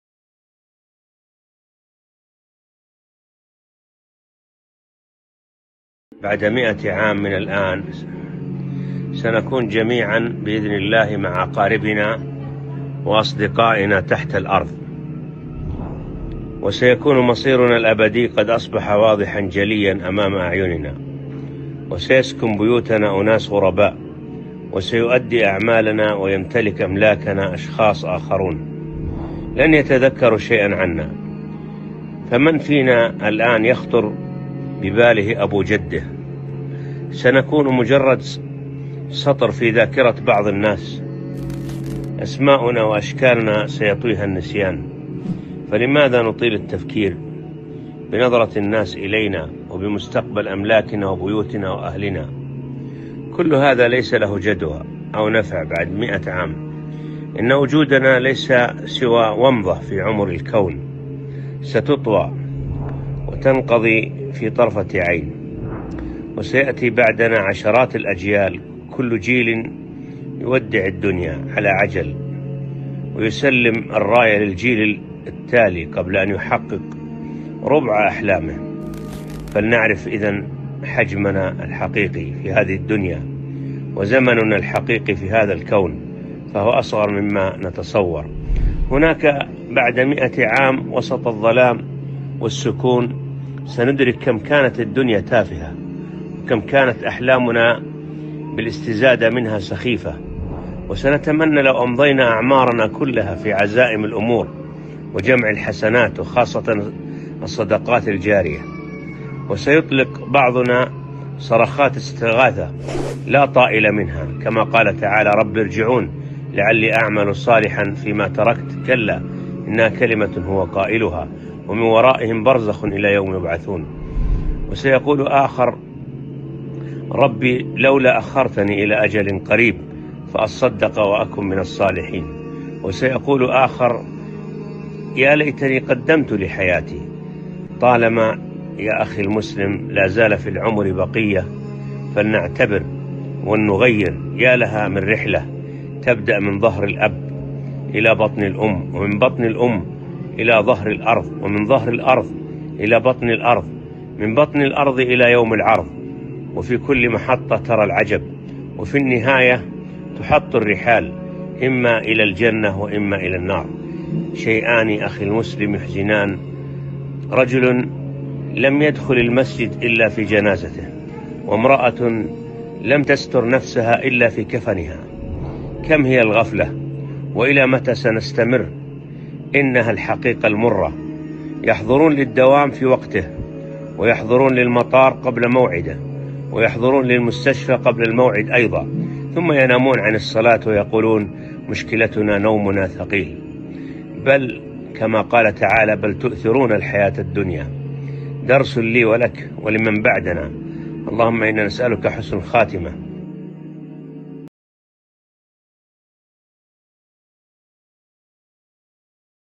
A HUNDERED YEARS FROM NOW | Inspiring Video | Who ARe You Living Your LIfe For?
A HUNDERED YEARS FROM NOW
WARNING: THIS VIDEO WILL CHANGE YOUR LIFE FOREVER
OUR EXISTENCE IS NOTHING BUT A FLASH IN THE AGE OF THE UNIVERSE THAT WILL FOLD AND PASS IN THE BLINK OF AN EYE
LET US KNOW OUR REAL WORTH IN THIS WORLD, AND OUR REAL TIME IN THIS UNIVERSE WHICH IS SMALLER THAN WE IMAGINE
The Bitter Truth
Who ARe You Living Your LIfe For?
Wake up Before it Becomes Too Late To Regret
After You Die, Who will Make Your Lord Please With You On Your Behalf?